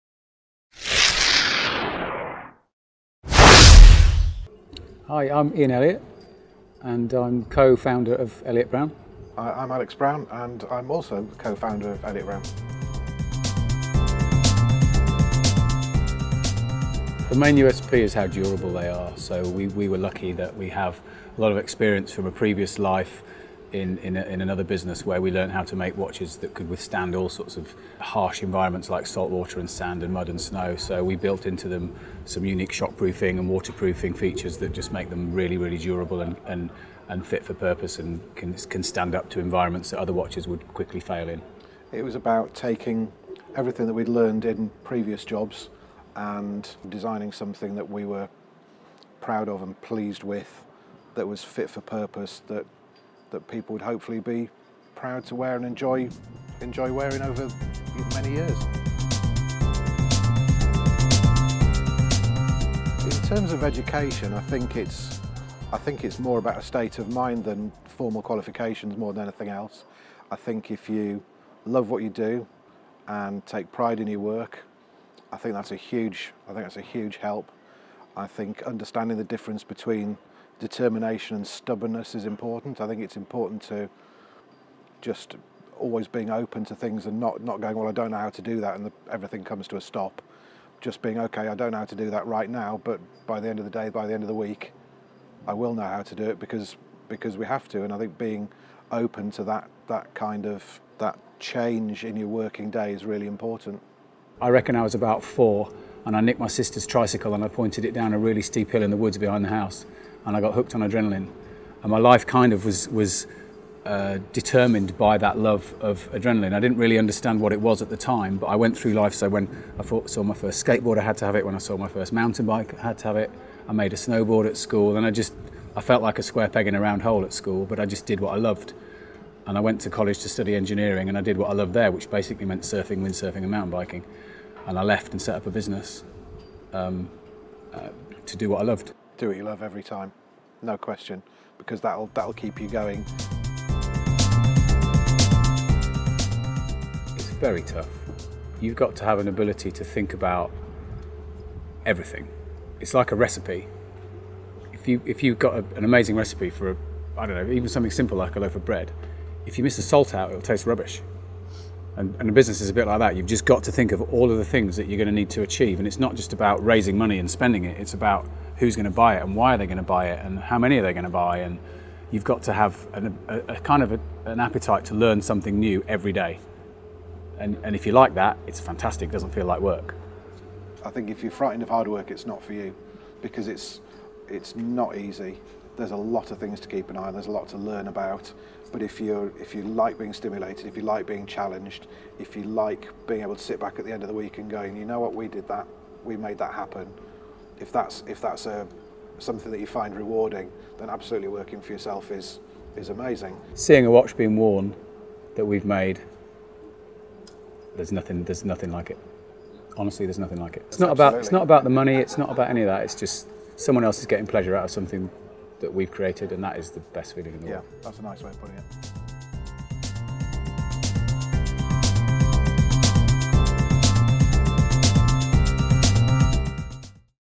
Elliot Brown on Engineering Entrepreneurship - Anturus Gear Series
The team at Elliot Brown talk about engineering the perfect time piece and why the best entrepreneurs never cut corners...

Anturus Education make educational videos to bring the science and tech of the outdoors into homes and classrooms!